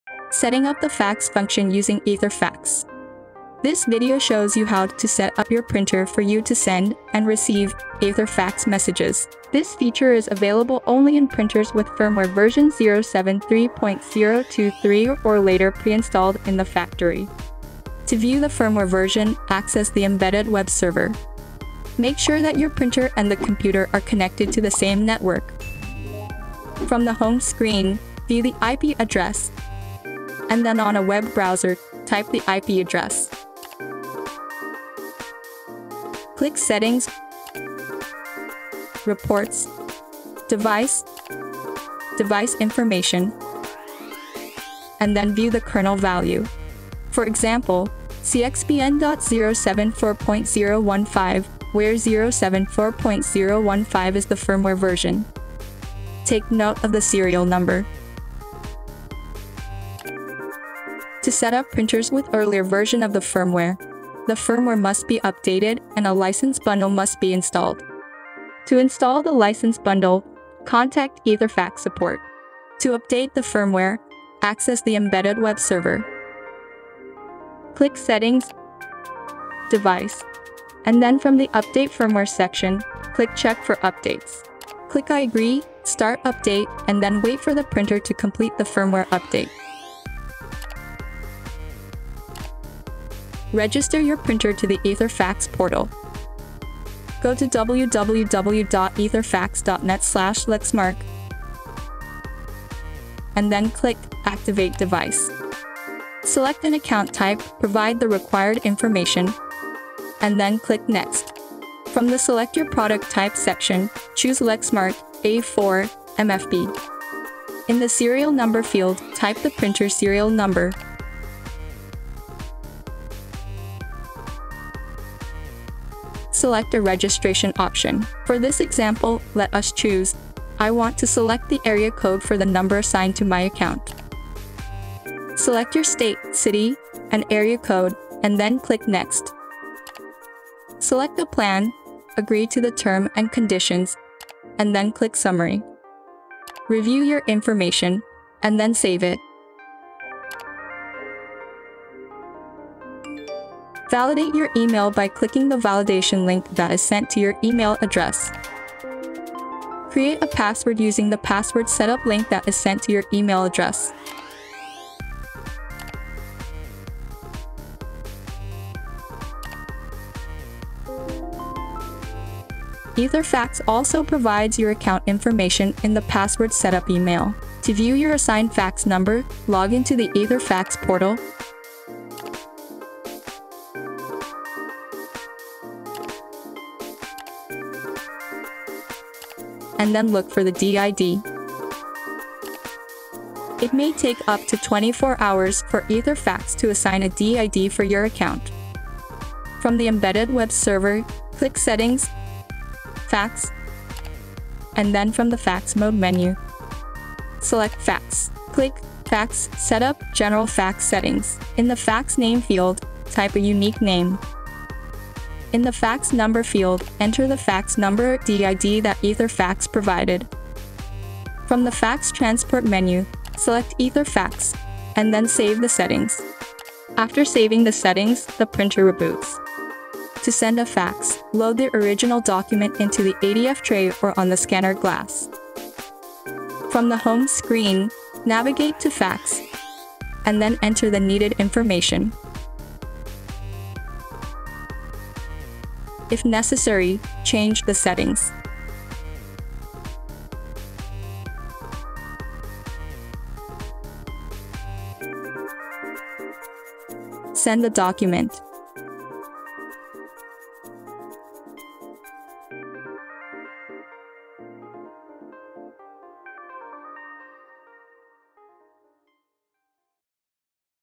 Lexmark—Setting up fax function using etherFAX
Configure the etherFAX settings for you to use the printer in sending and receiving fax messages.

This feature is available in printers with firmware version 073.023 or later pre-installed in the factory.

00:00 Introduction
00:09 Firmware information
00:18 View firmware version of your printer
01:04 Updating firmware
01:34 Register your printer to etherFAX portal
02:57 View your assigned fax number from etherFAX and input it on the Embedded Web Server
03:50 Sending fax